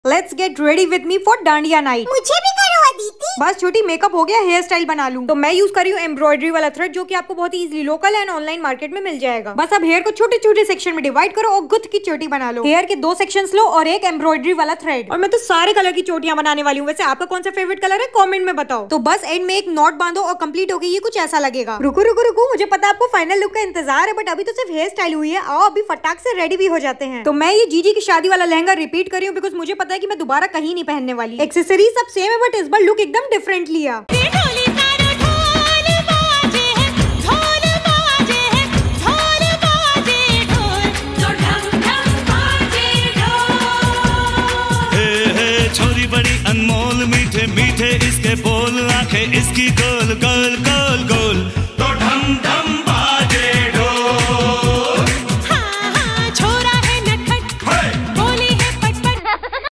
GRWM for Dandiya Night😱
I'm back with fun Hairstyle. Let's Get Ready With Me for Dandiya Night. This Colourful Braid is so amazing and I personally enjoyed the process. Do recreate and share your creation with me on my Instagram Page and get featured.